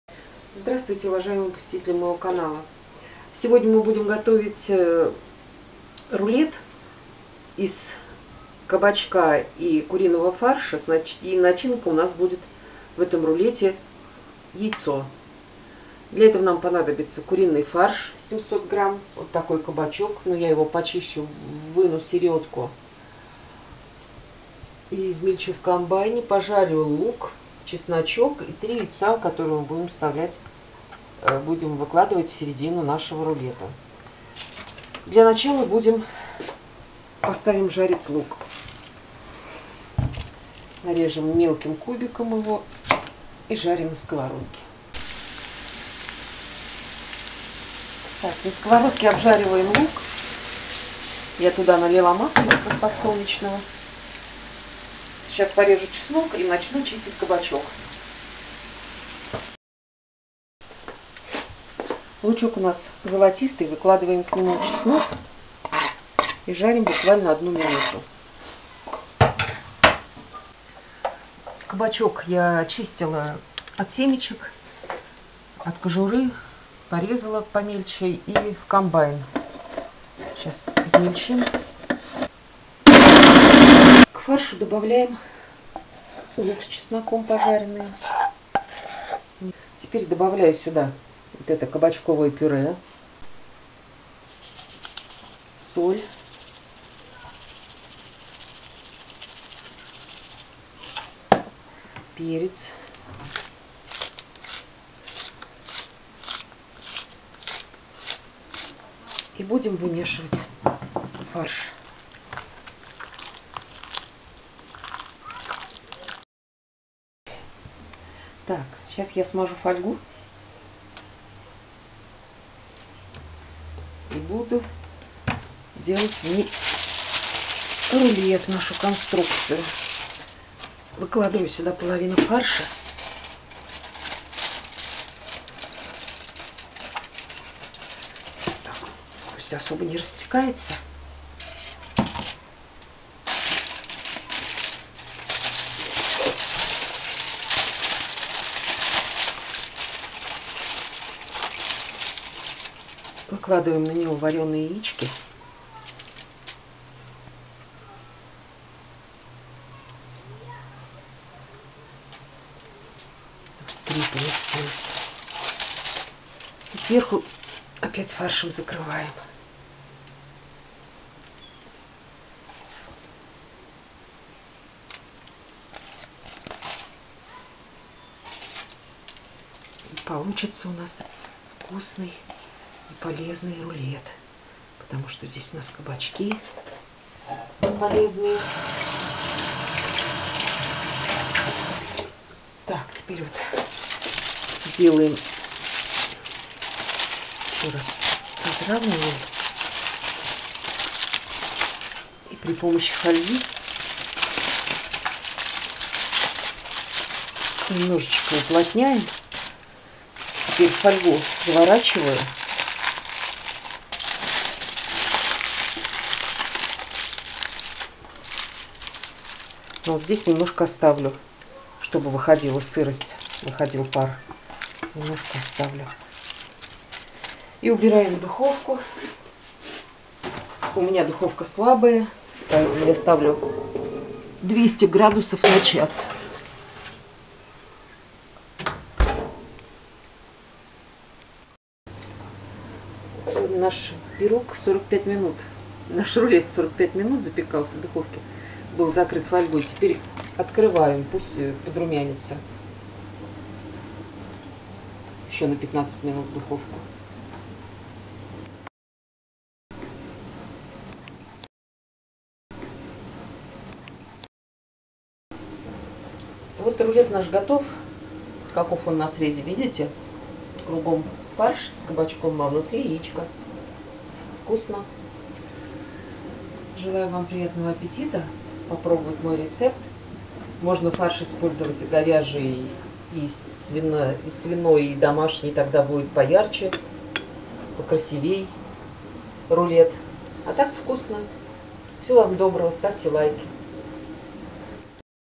Кабачково - куринный рулет с яйцом
Не знаете, что ещё приготовить с кабачками? Попробуйте этот рецепт
Понадобится 1 кабачок
1-2 луковицы
1 зубчик чеснрка
700 граммов фарша
масло растительное
3 вареных яйца